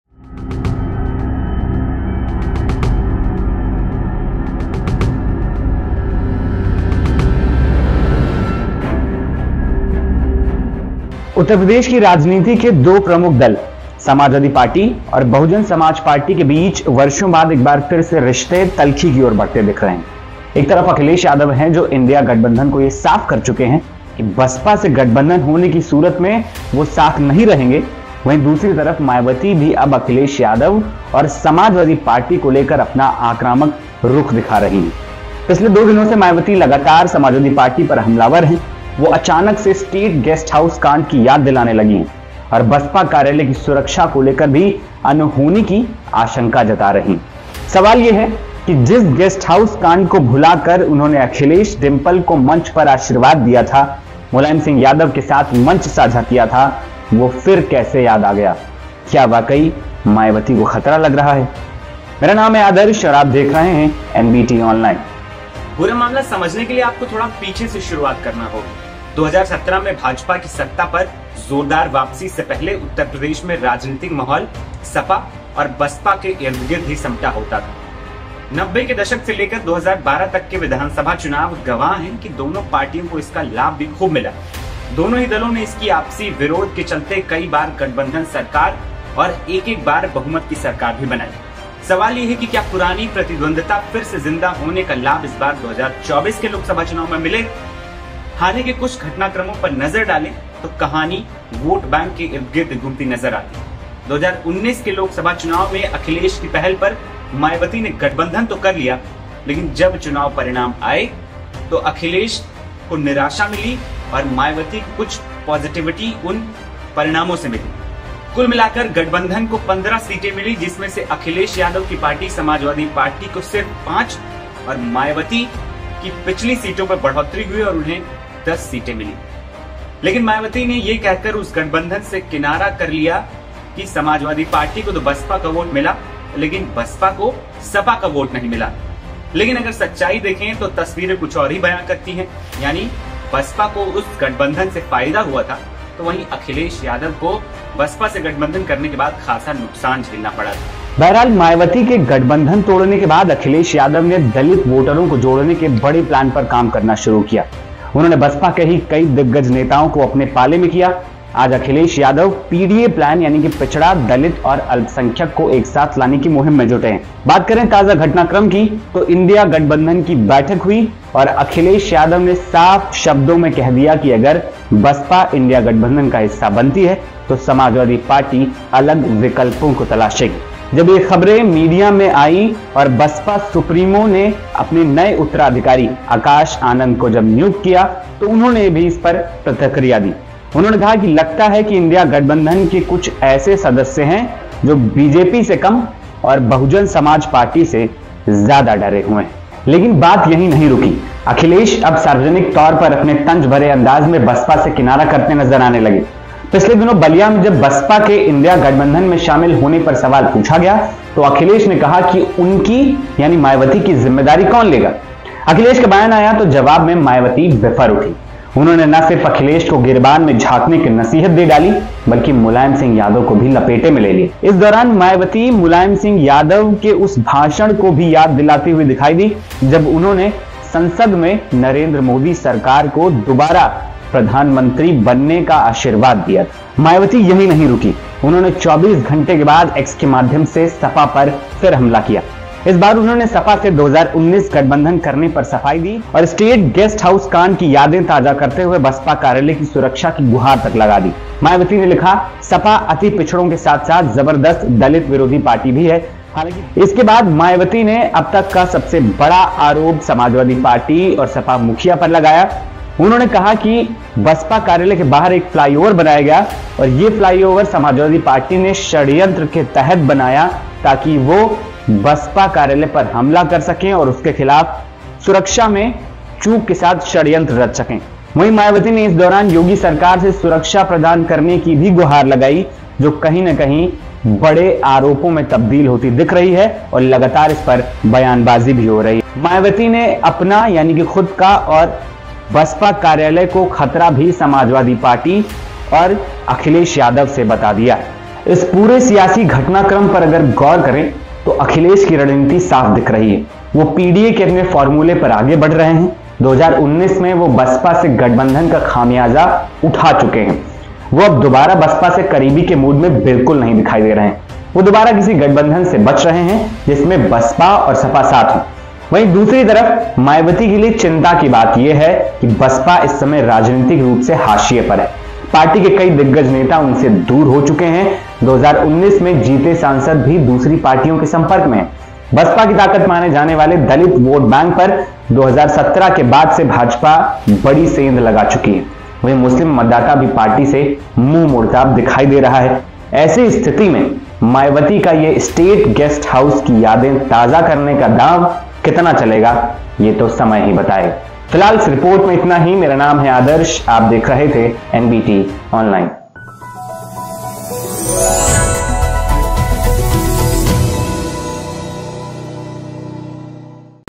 Maya को क्यों आई गेस्ट हाउस कांड की याद? Akhilesh और Mayawati के बीच तल्खियों का गणित समझिए | NBT UP
उत्तर प्रदेश की राजनीति के दो प्रमुख दल समाजवादी पार्टी और बहुजन समाज पार्टी के बीच वर्षों बाद एक बार फिर रिश्ते तल्खी की ओर बढ़ते दिख रहे हैं। एक तरफ अखिलेश यादव हैं जो इंडिया गठबंधन को साफ कर चुके हैं कि बसपा से गठबंधन होने की सूरत में वह साथ नहीं रहेंगे। वहीं दूसरी तरफ मायावती भी अब अखिलेश यादव और समाजवादी पार्टी को लेकर अपना आक्रामक होती दिख रही हैं। पिछले दो दिनों से मायावती लगातार समाजवादी पार्टी पर हमलावर हैं। वह अचानक से स्टेट गेस्ट हाउस कांड की याद दिलाने लगी हैं और बसपा कार्यालय की सुरक्षा को लेकर अनहोनी की आशंका जता रही हैं। सवाल ये है कि जिस गेस्ट हाउस कांड को भुलाकर उन्होंने अखिलेश-डिंपल को मंच पर आशीर्वाद दिया था, मुलायम सिंह यादव के साथ मंच साझा किया था, वह फिर कैसे याद आ गया? क्या वाकई मायावती को खतरा लग रहा है?